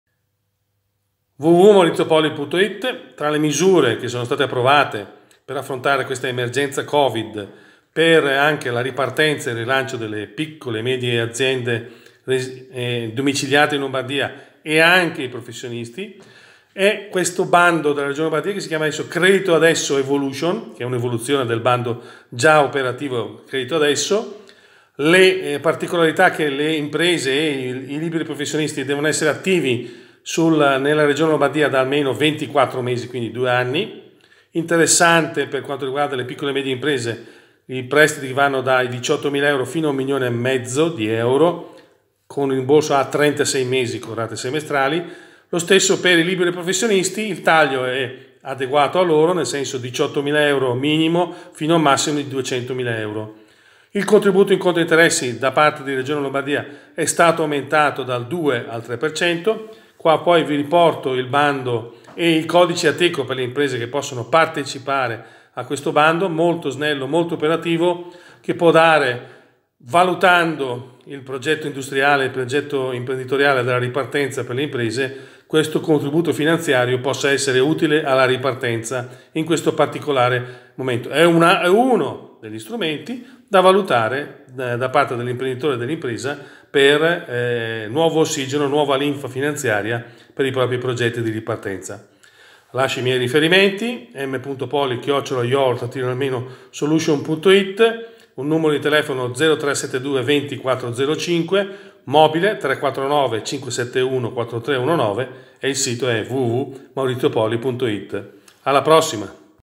Credito Adesso Evilution: Agevolaziono per Pmi e Professionisti
creditoadessoevolution evolution Credito Adesso Evolution Alle piccole e medie imprese che hanno bisogno in tempi rapidi di liquidità per ripartire Finlombarda propone la misura Credito Adesso Evolution La misura approvata per affrontare l’emergenza Covid-Credito19, un’iniziativa pensata per soddisfare in tempi rapidi l’esigenza di piccole e medie imprese e professionisti lombardi di disporre di liquidità per garantire la continuità aziendale e ripartire.